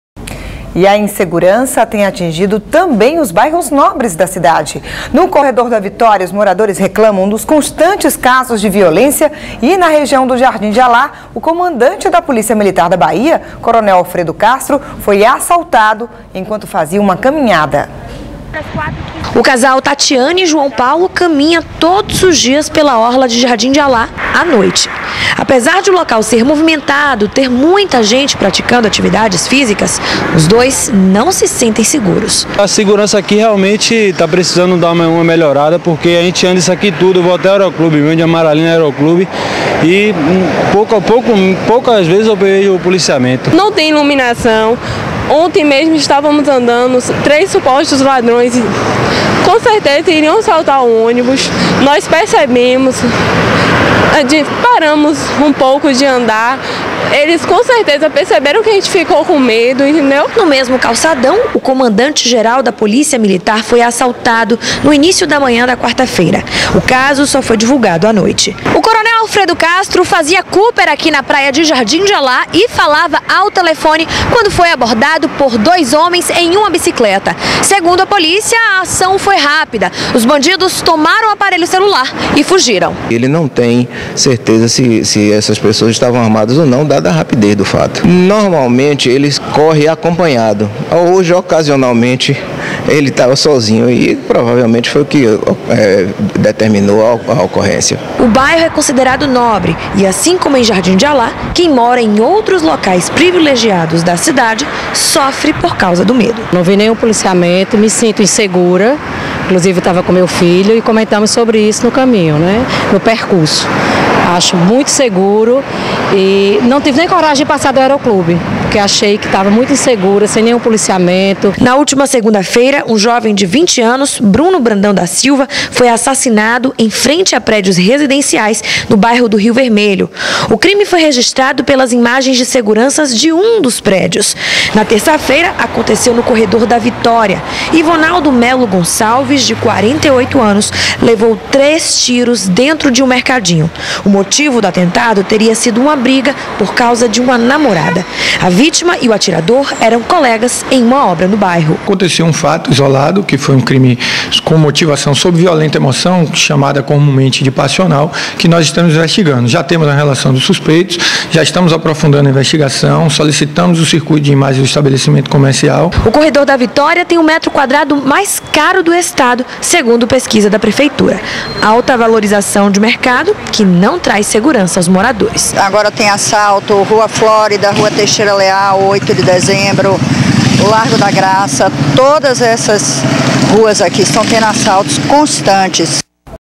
Insegurança cresce em bairros nobres de Salvador
No Corredor da Vitória, moradores reclamam dos constantes casos de violência na região.